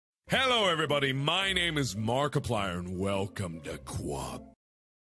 Markiplier Test Animation
Quick animation test of a Markiplier puppet built by me.